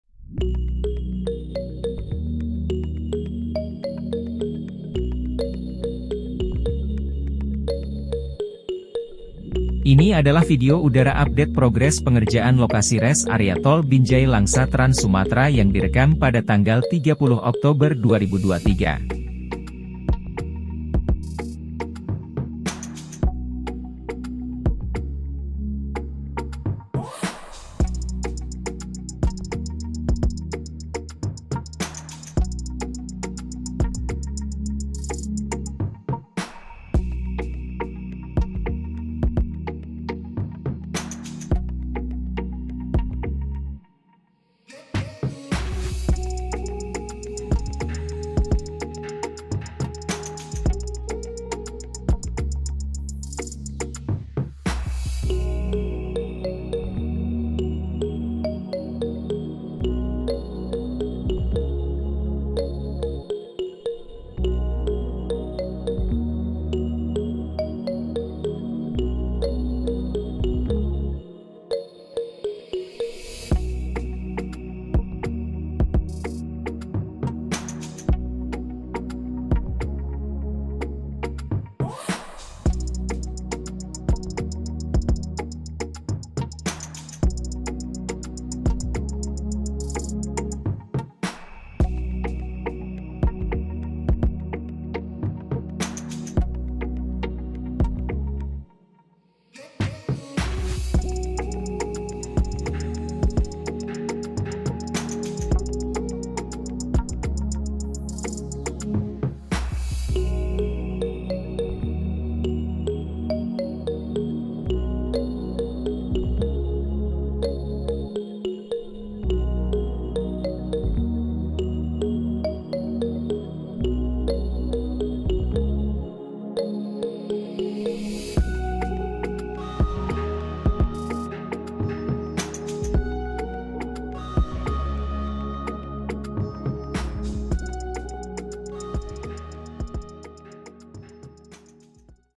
UPDATE REST AREA TOL BINJAI-LANGSA TRANS SUMATERA 30 OKTOBER 2023
UPDATE REST AREA TOL BINJAI-LANGSA TRANS SUMATERA 30 OKTOBER 2023.Video udara update progres pengerjaan lokasi rest area tol binjai langsa jalan tol sumut-aceh trans sumatera yang direkam pada tanggal 30 oktober 2023 by drone video. Travel berita terbaru news restarea tolbinjailangsa transsumatera tolmedanaceh toltranssumatera @MasbroTutorialOfficial